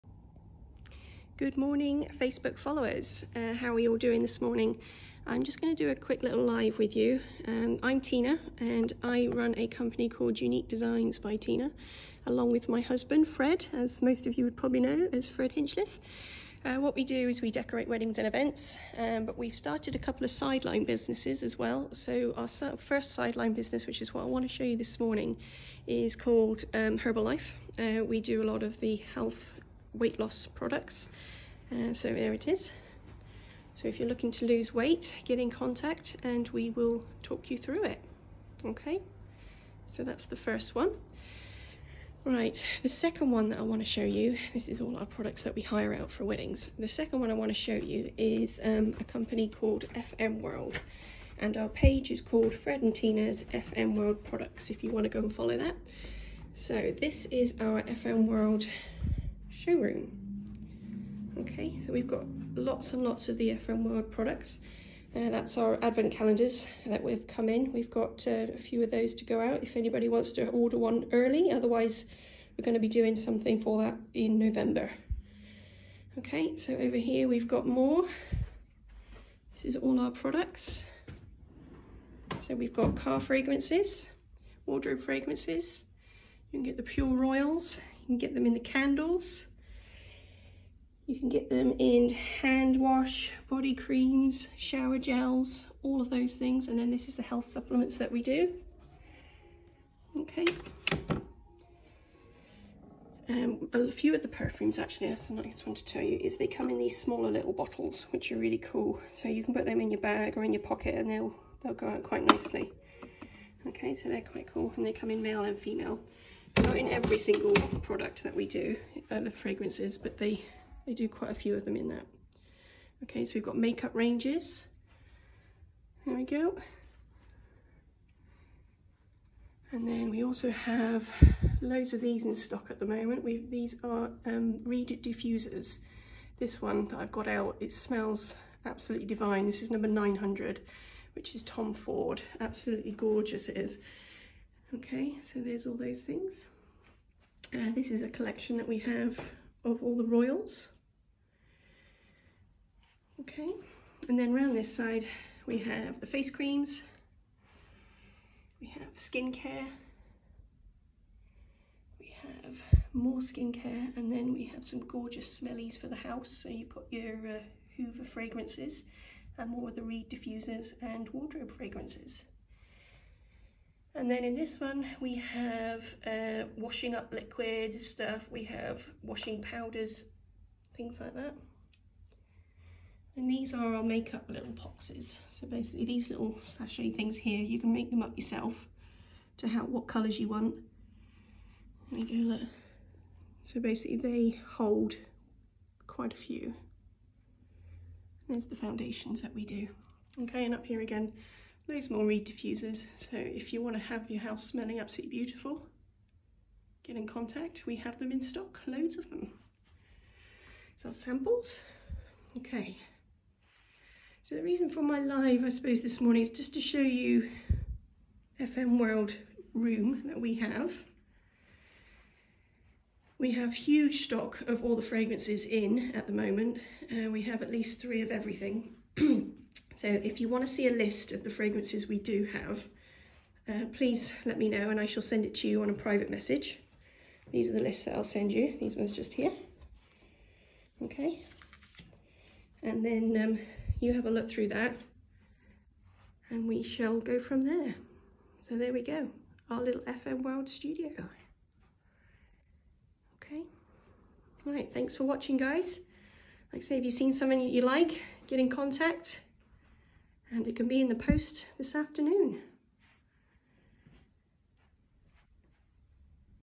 Quick little video
Short video of our showroom and a chat about our new sideline companies